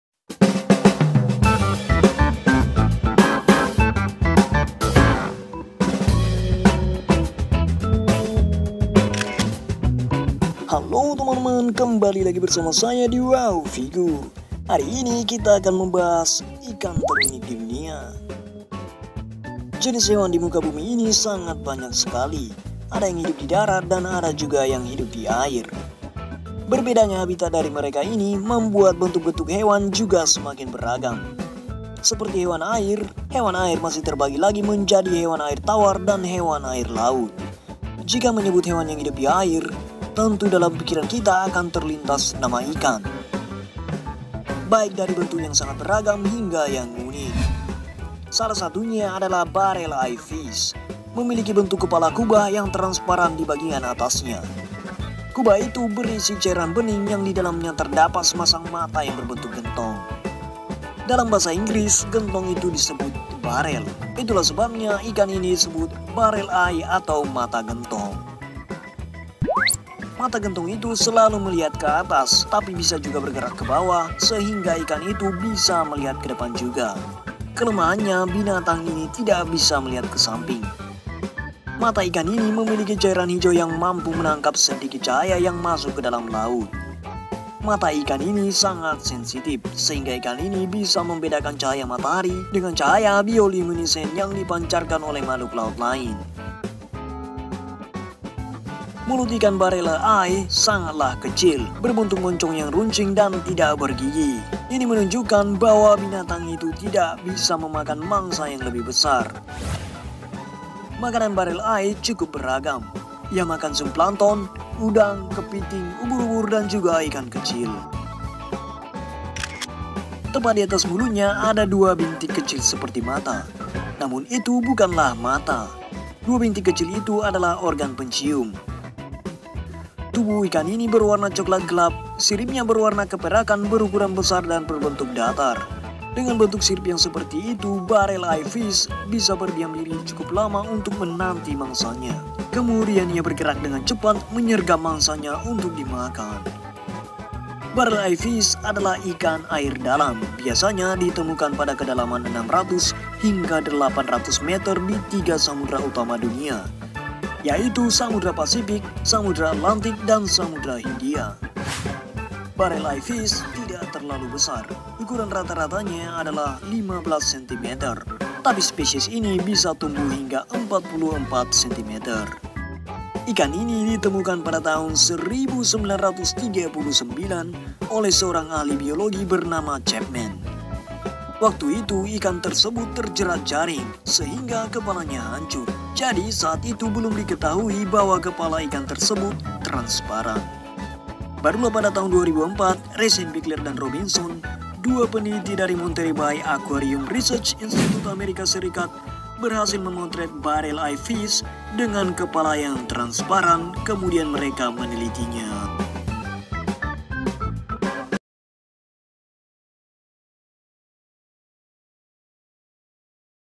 Inilah Ikan Terunik Di Dunia, dengan kepala TRANSPARAN
Barreleye fish memiliki kepala berbentuk kubah yang transparan di bagian atasnya. “Kubah” itu berisi cairan bening dan di dalamnya terdapat sepasang mata berbentuk gentong.

Dalam bahasa Inggris, gentong itu disebut barrel. Itu sebabnya ikan ini disebut barrel eye atau mata gentong.

Mata gentong itu selalu melihat ke atas, tapi bisa juga bergerak ke bawah, sehingga ikan itu bisa juga melihat ke depan. Kelemahaannya, binatang ini tidak bisa melihat ke samping.

Mata ikan ini memiliki cairan hijau yang mampu menangkap sedikit cahaya yang masuk ke kedalaman laut.
Mata ikan ini sangat sensitif sehingga ikan ini bisa membedakan cahaya matahari dengan cahaya bioluminescent yang dipancarkan oleh makhluk laut lainnya yang merupakan mangsanya.